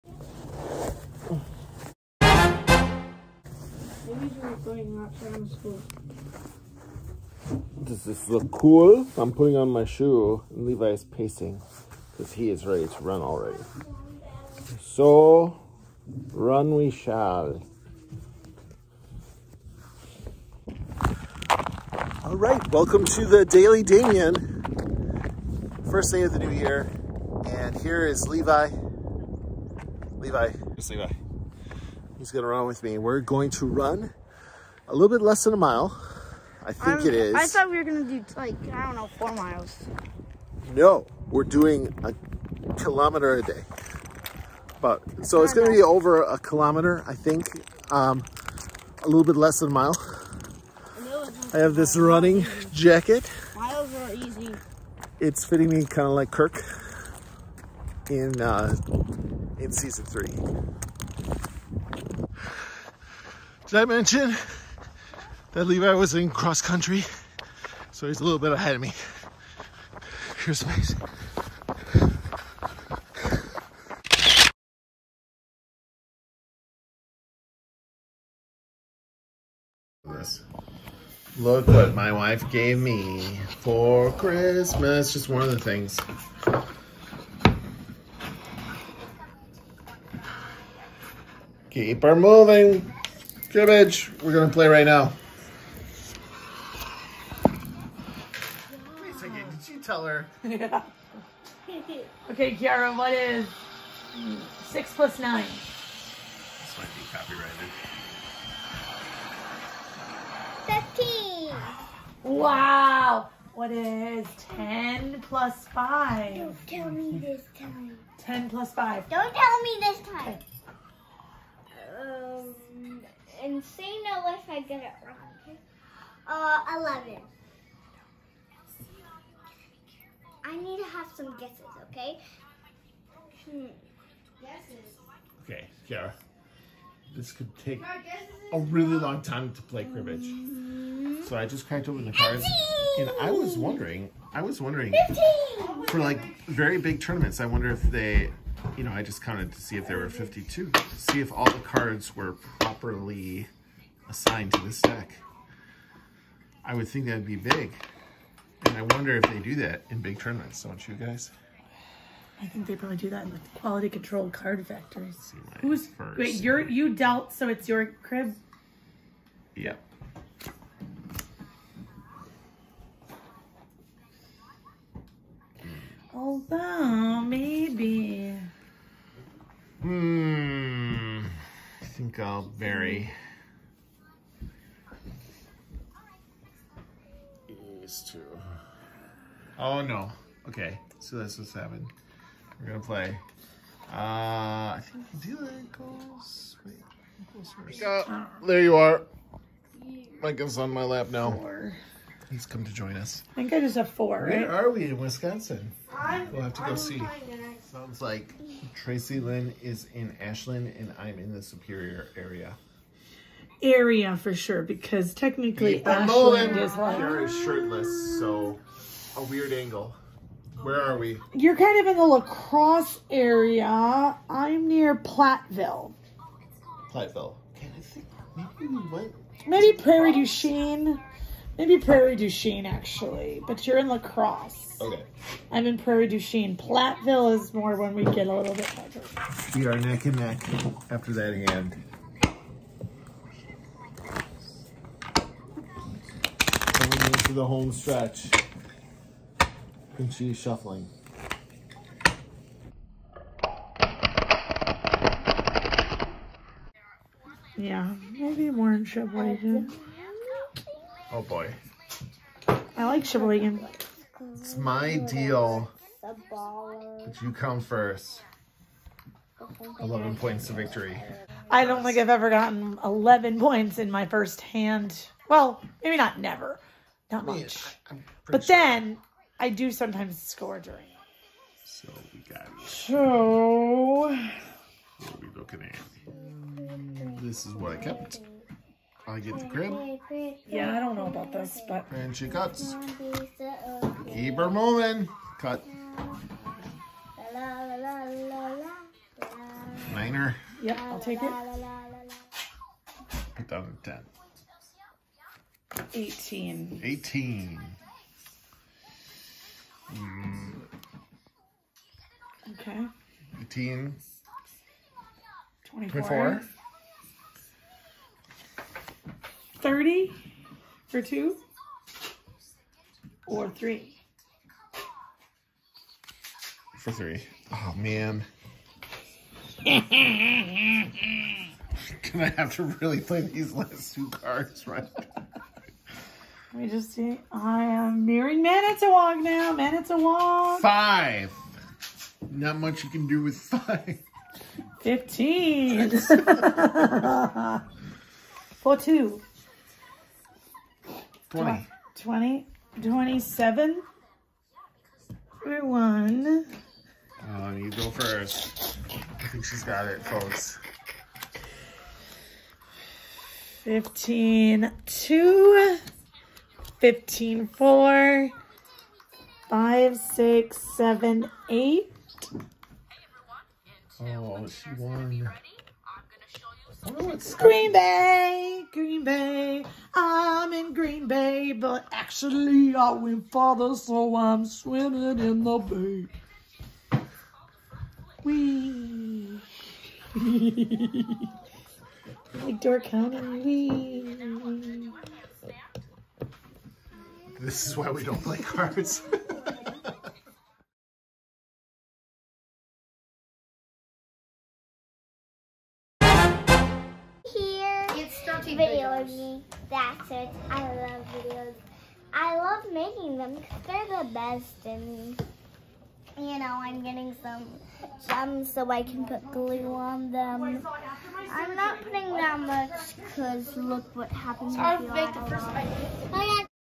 1K-a-day in 2022 and new Cribbage set!
Trying to make the Daily D daily in 2022. I’ve been slowly taking off the weight at the end of 2021 with better eating practices with Noom but no reach uptick in physical activity. I figure a run around my block is 1.4 km. And 1K-per-day is a good challenge and a good base. Also cracked open my new cribbage set which was a Christmas present from my wife. I even dealt first and she beat me!

I hope 2022 is off to a good start for you! Any resolutions?